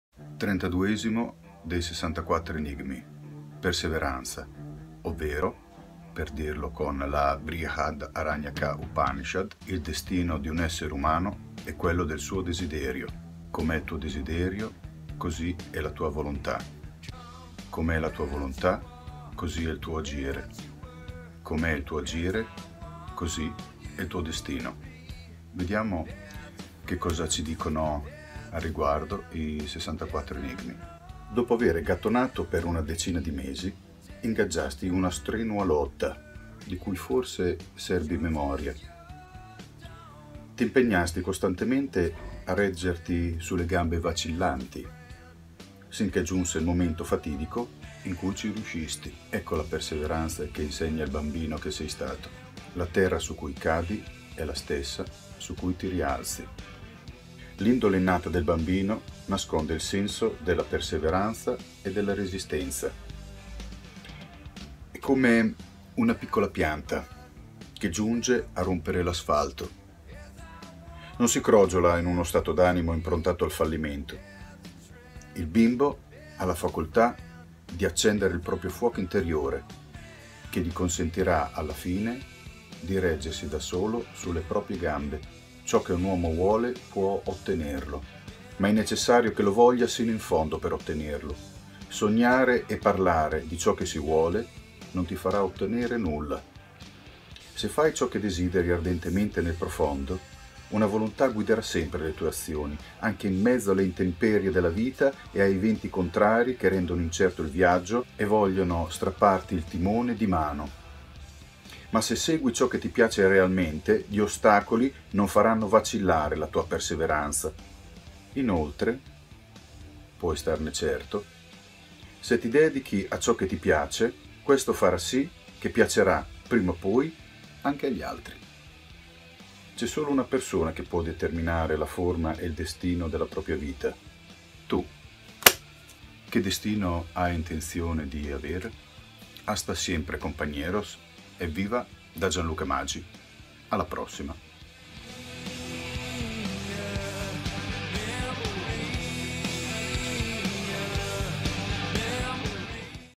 Che destino hai deciso di avere?
Il destino di un essere umano è quello del suo desiderio: com’è il tuo desiderio, così è la tua volontà; com’è la tua volontà, così il tuo agire; com’è il tuo agire, così il tuo destino.
da: Gianluca Magi, 64 ENIGMI. Qui ☞

 L’EROS PURO DEL GIOCO DELL’EROE
Quello che più colpisce, ogni volta, è l’intesa che unisce i Giocatori facendo di loro un solo corpo.
Una Forza a più teste. Che, al contempo, realizza il miracolo di esaltare le differenze di tutte le persone in Gioco.
Questa è stata l’atmosfera di domenica 14 maggio nell’Atelier del M° Luigi Scapini per il 6° Livello del Gioco dell’Eroe, giornata organizzata con grande rinnovata ospitalità e amicizia da Paola.
Grazie di cuore a tutti, uno ad uno per la giornata NON DUALE.
Evviva ai GdE Player!

★ GIANLUCA MAGI MYSTERY (s&v) TOUR mag. 2017